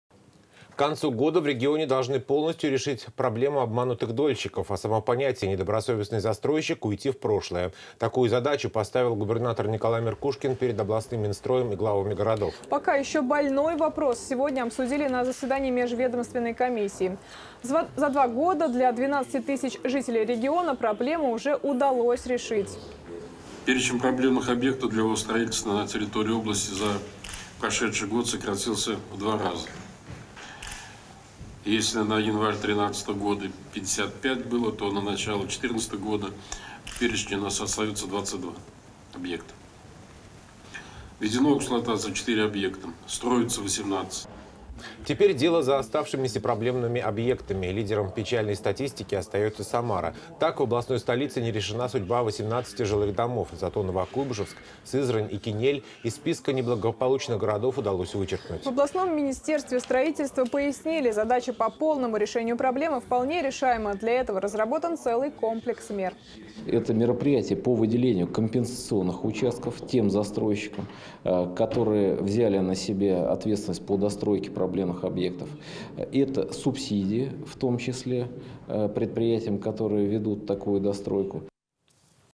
Заседание по долевому строительству
К концу года в регионе должны полностью решить проблему обманутых дольщиков, а само понятие недобросовестный застройщик - уйти в прошлое. Такую задачу поставил губернатор Николай Меркушкин перед областным минстроем и главами городов.  Пока еще больной вопрос сегодня обсудили на заседании межведомственной комиссии. За два года для 12 тысяч жителей региона проблему уже удалось решить.